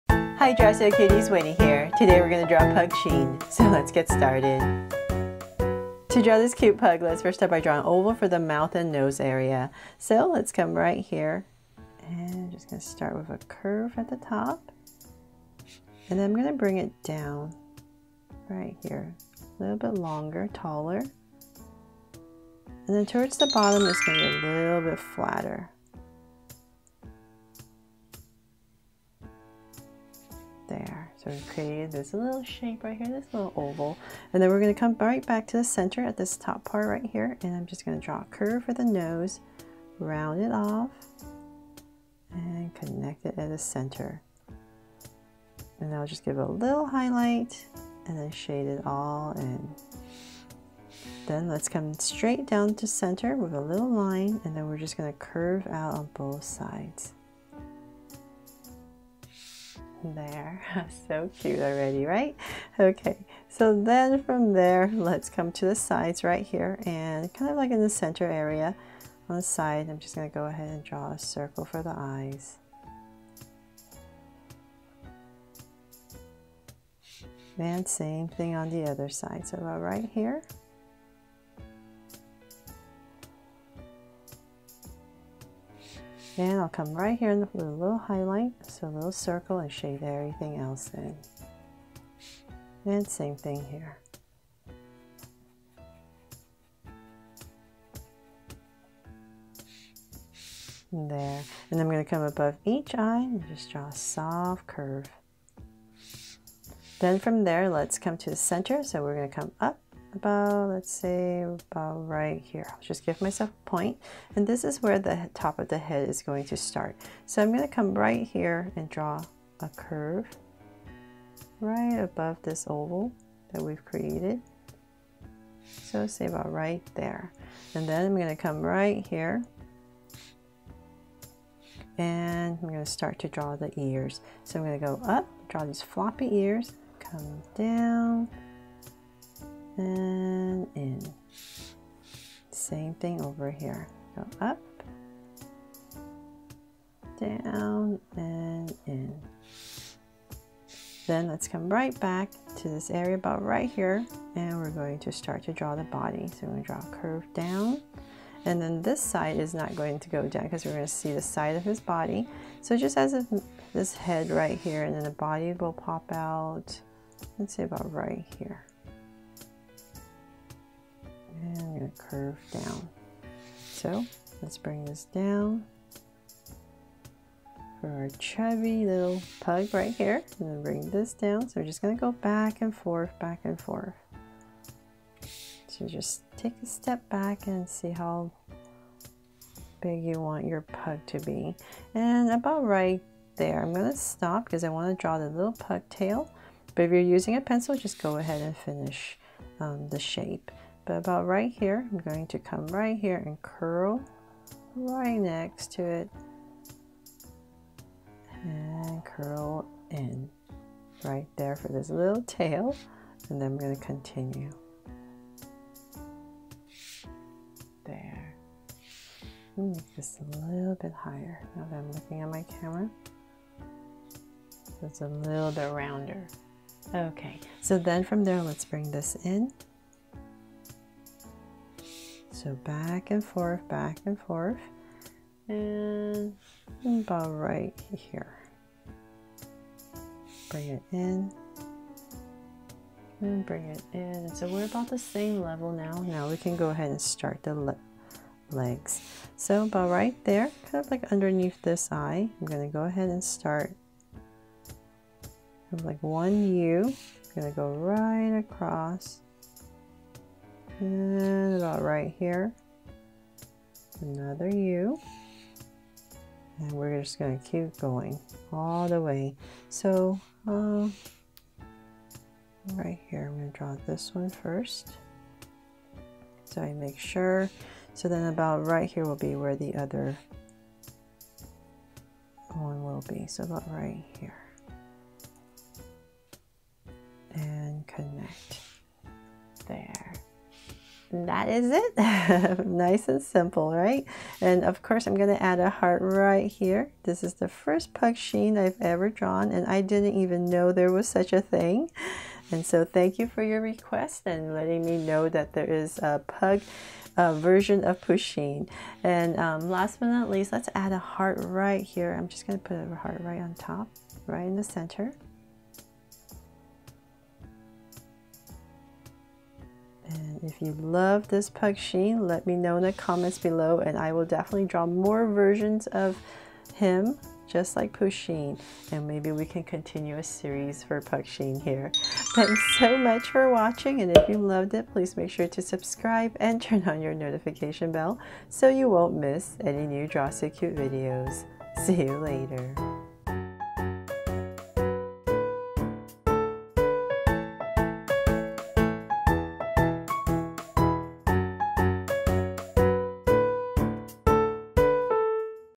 How to Draw a Pug Easy 🦴Pugsheen
Learn How to Draw a cute Pug Puppy Dog easy inspired by Pusheen's Pugsheen. Step by step cartoon dog drawing.